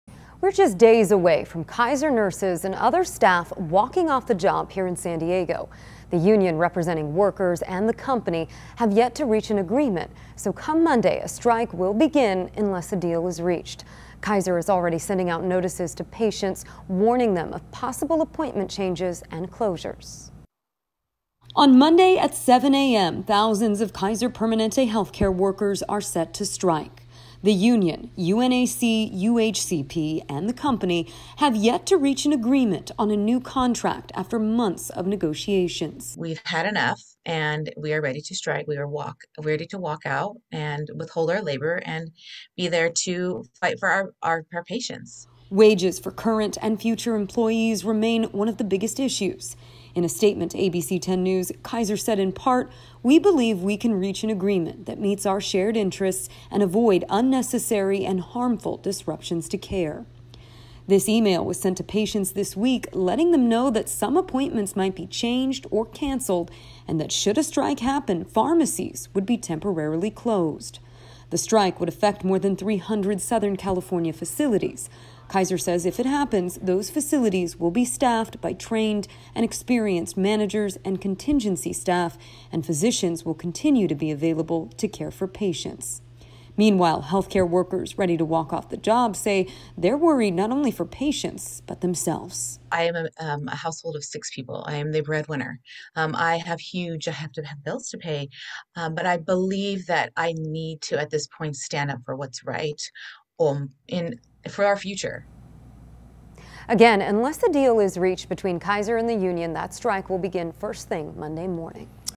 San Diego Kasier health care workers prepare to strike
On Monday at 7 a.m. thousands of Kaiser Permanente health care workers are set to strike.